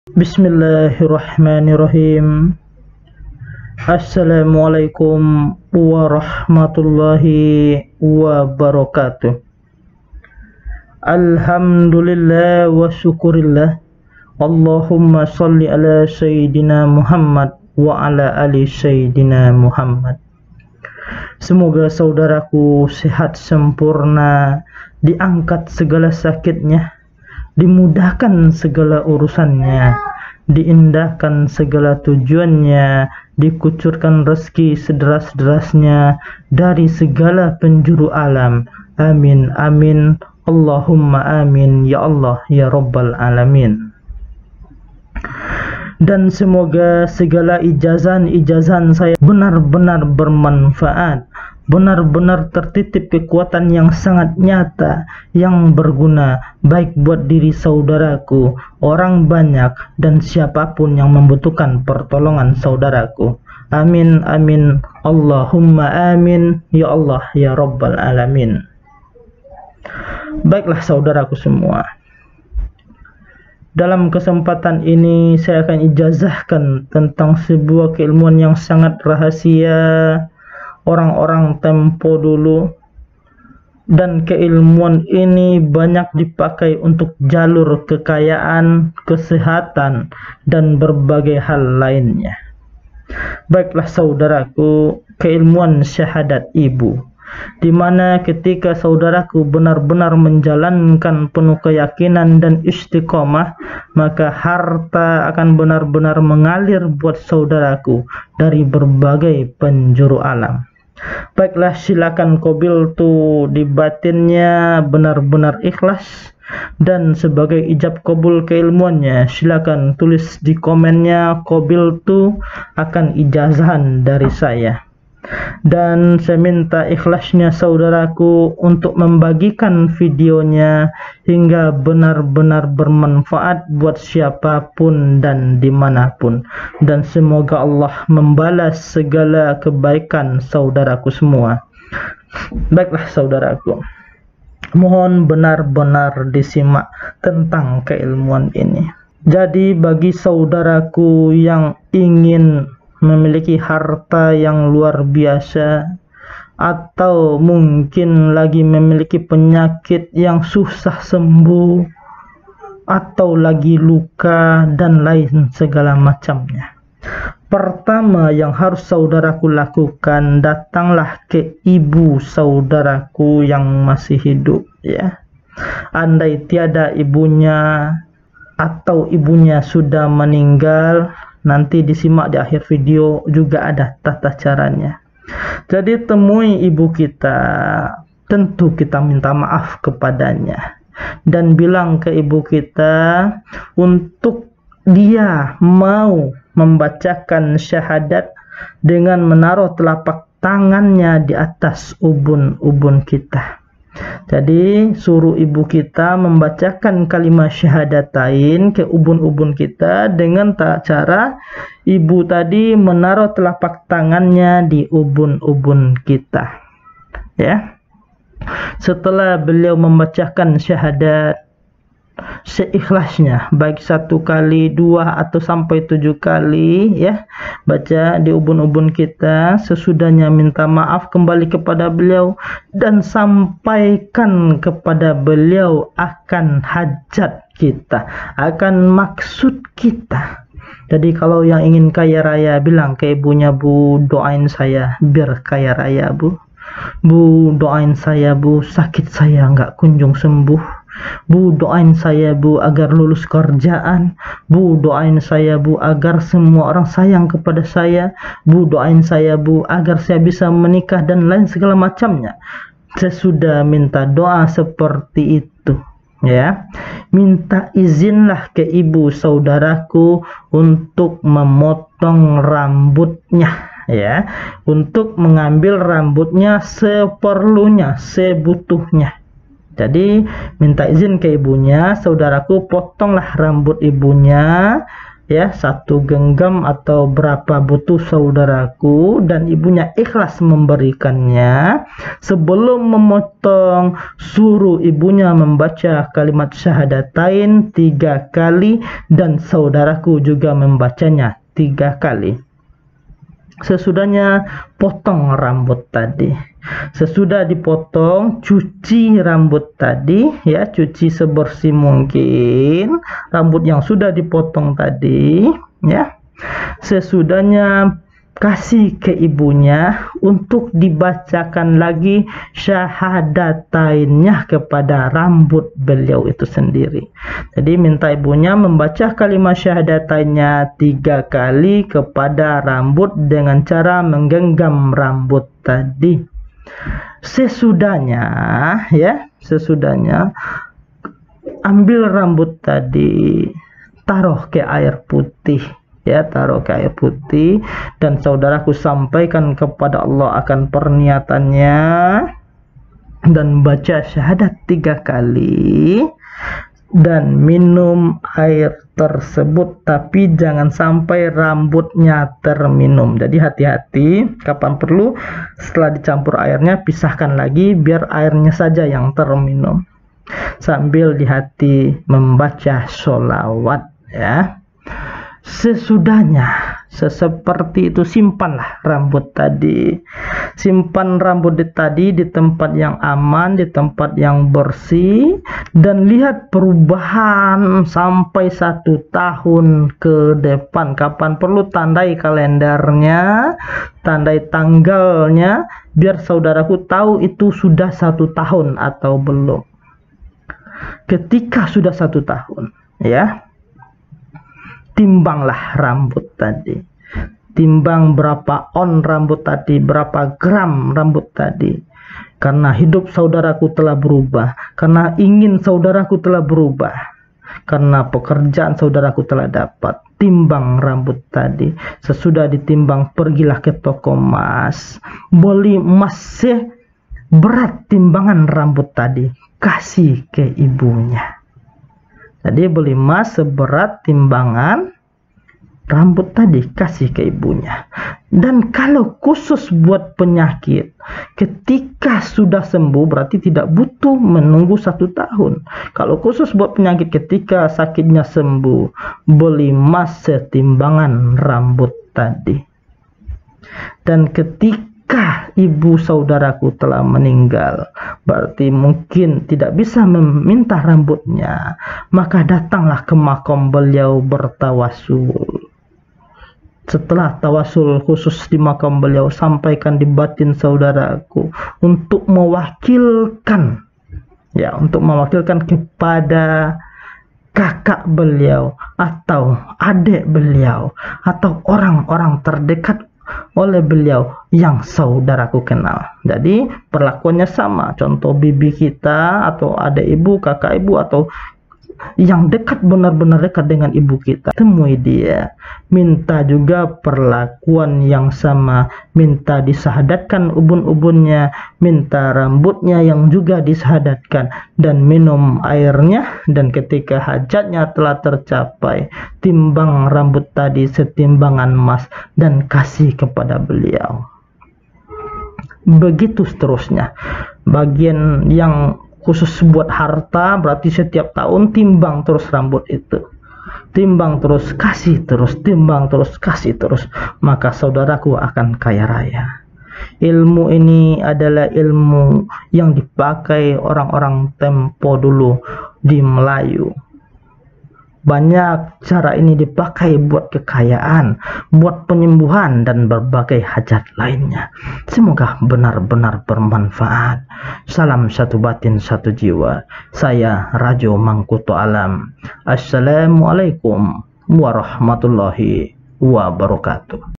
ILMU BUAT KEKAYAAN, ILMU SYAHADAT IBU, TAJRIB, CUKUP DILAKUKAN 1 X SETAHUN
Silahkan benar benar di simak vidionya, hingga benar benar paham